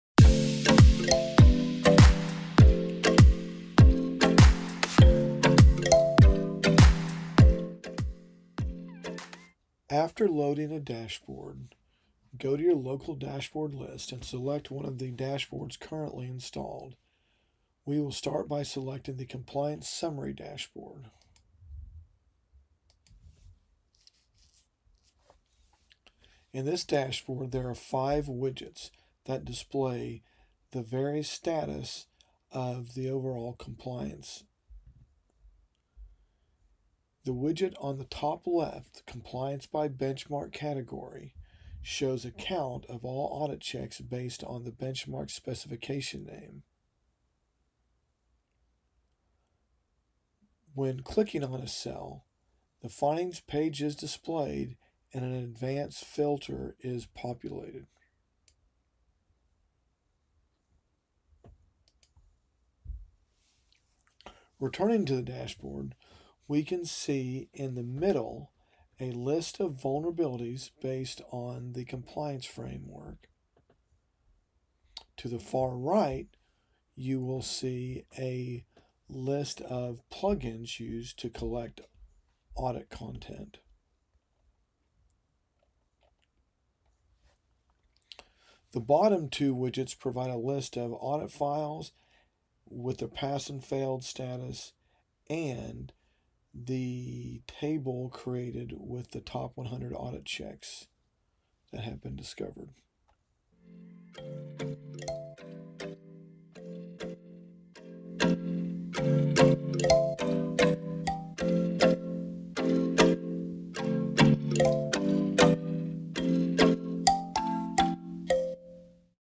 Understanding the Compliance Summary Dashboard
In this video, we dissect the Compliance Summary Dashboard, a tool for monitoring compliance status. Learn how to navigate through its widgets, including Compliance by Benchmark Category and vulnerability lists based on compliance frameworks. Discover how to interact with findings, utilize advanced filters, and interpret plugin usage data. Gain insights into managing audit files, tracking pass/fail counts, and identifying top audit checks.